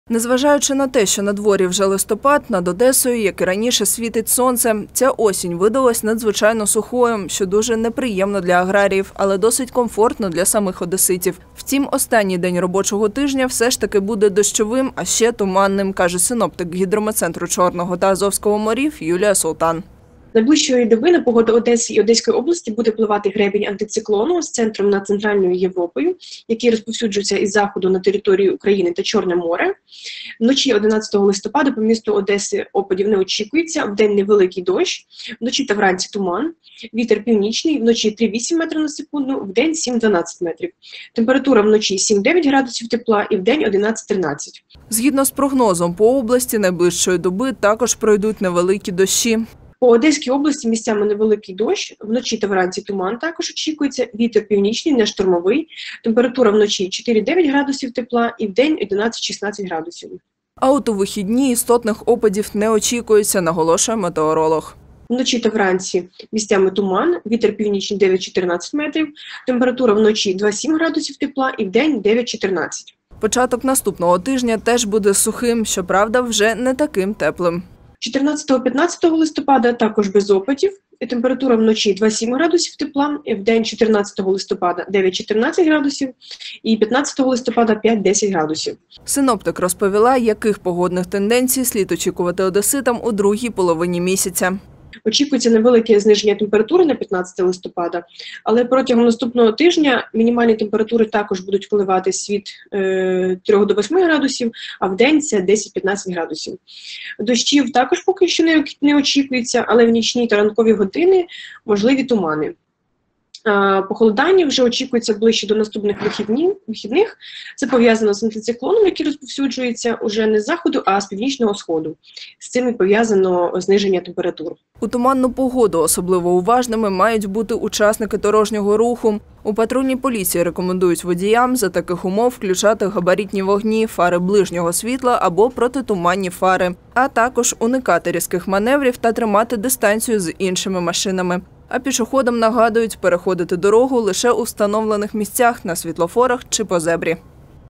Найближчої доби в Одесі прогнозую дощі
В Одесі ця осінь видалася надзвичайно сухою – вкрай неприємно для аграріїв, але досить комфортно для самих одеситів. Втім, останній день робочого тижня все ж таки буде дощовим, а ще – туманним, каже синоптик Гідрометцентру чорного та азовського морів Юлія Султан.